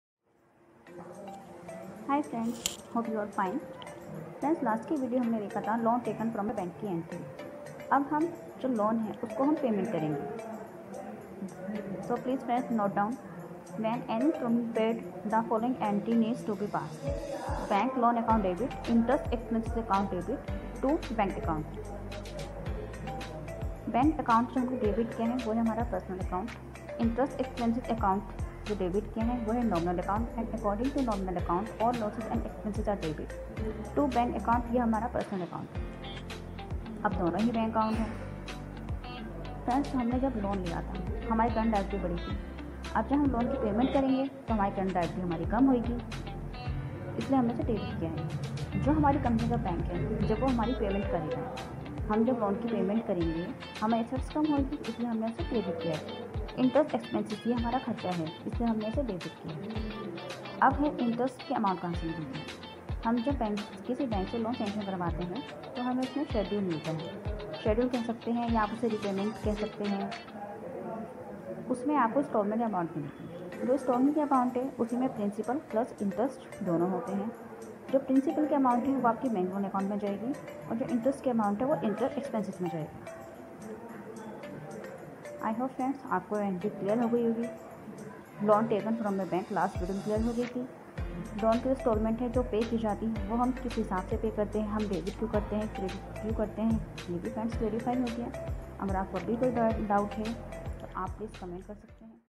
How to pass entry of Repayment of loan with golden rules of accounting
Traditional rules applied
Clarification of Debit & Credit